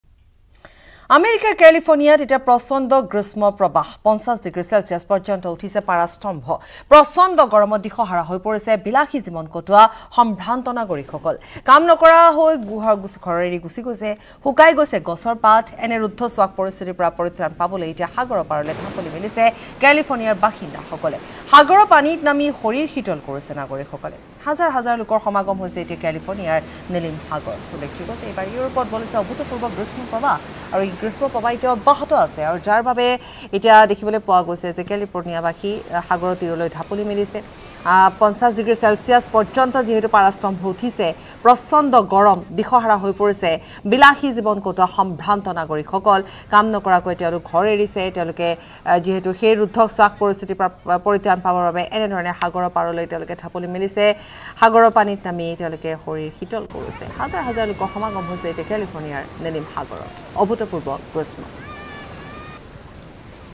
California's Heat Wave : দিশহাৰা হৈ পৰিছে বিলাসী জীৱন কটোৱা নাগৰিক | Assamese News
California's Heat Wave | Americaৰ Californiaত প্ৰচণ্ড গ্ৰীষ্ম প্ৰবাহে দহিছে । গৰমৰ পৰা পৰিত্ৰান পাবলৈ সকলো ঢাপলি মেলিছে সাগৰৰ পাৰলৈ

 News18 Assam Northeast | Assam News | News in Assam | Assamese News | Northeast news | Northeast India | n18oc_world